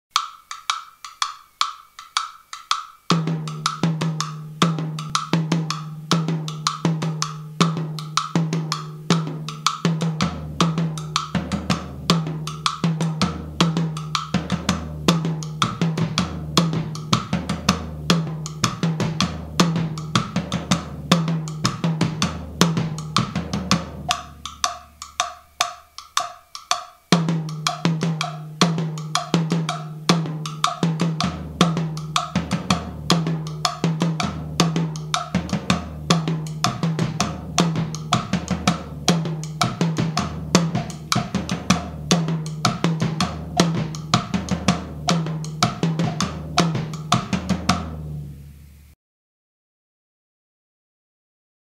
La Independencia de la Clave 2: Makuta
Hola! Te presento la Makuta (ejercicio 60 a 68) de mi libro "La Independencia de la Clave Vol 2".
RH katá
LH Cachimbo, Mula
RF Caja
LF Guataca
Si te interesa el libro escribime! Gracias!

Hello! Here it is the Makuta (exercise 60 to 68) of my book "La Independencia de la Clave Vol 2"
RH katá
LH Cachimbo, Mula
RF Caja
LF Guataca
If you're interested in the book, write to me!